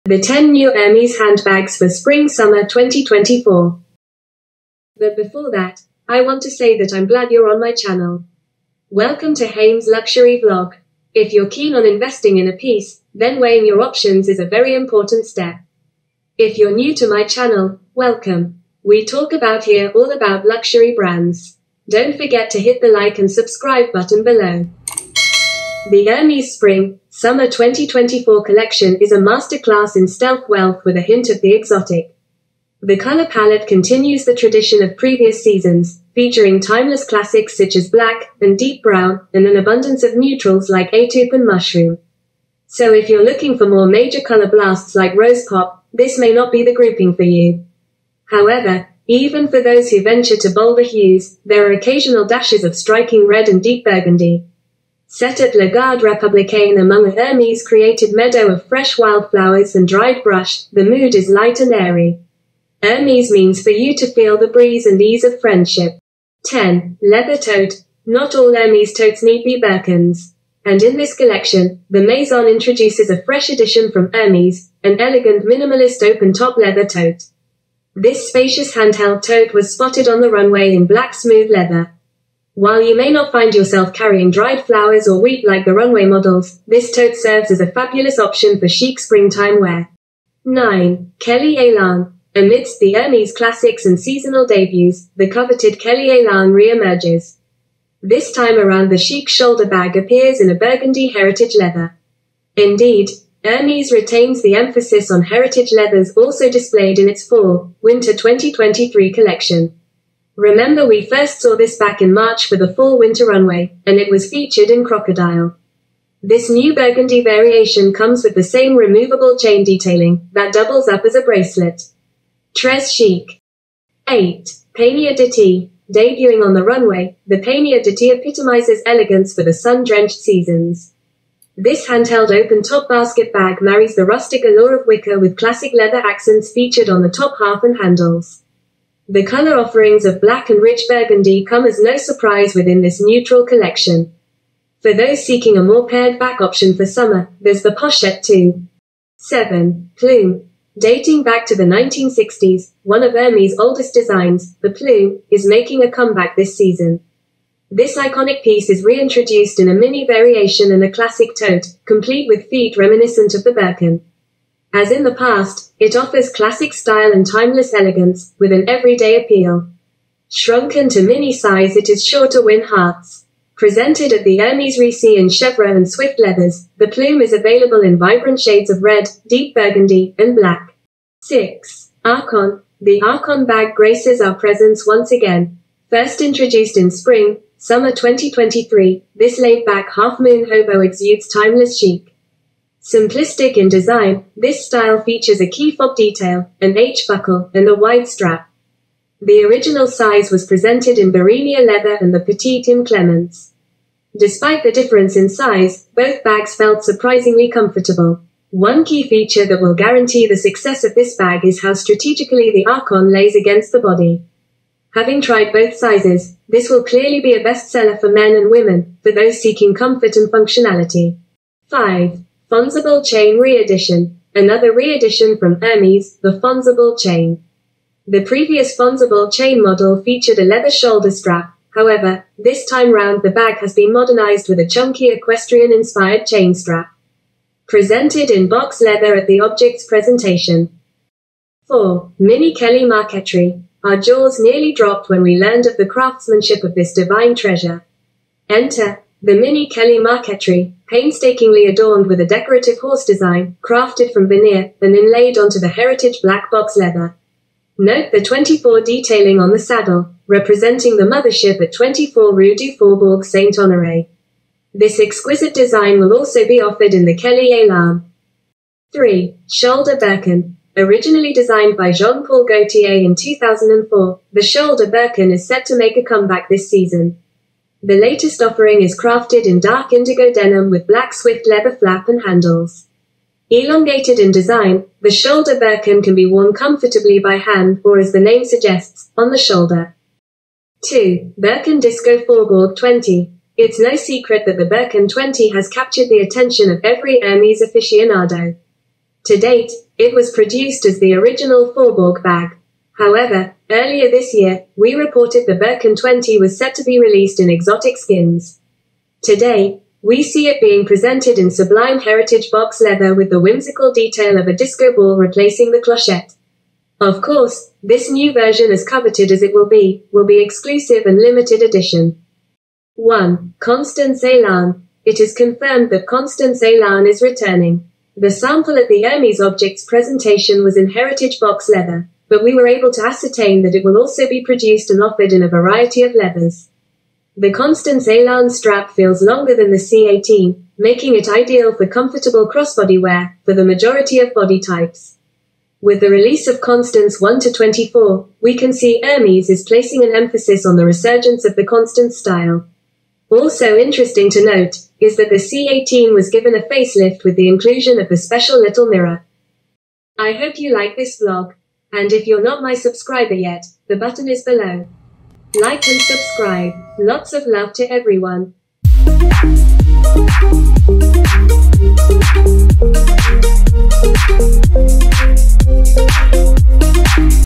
The 10 New Hermès Handbags for Spring Summer 2024 | Hymme's Luxury Vlog
Welcome to Hymme's Luxury Vlog.
The 10 New Hermès Handbags for Spring Summer 2024
10. Hermès Leather Tote
9. Hermès Kelly Elan
8. Hermès Panier d’été
7. Hermès Plume
6. Hermès Arçon
5. Hermès Fonsebelle Chaine Re-edition
4. Hermès Mini Kelly Marqueterie
3. Hermès Shoulder Birkin
2. Hermès Birkin Disco Faubourg 20
1. Hermès Constance Elan
Lot's of love to everyone!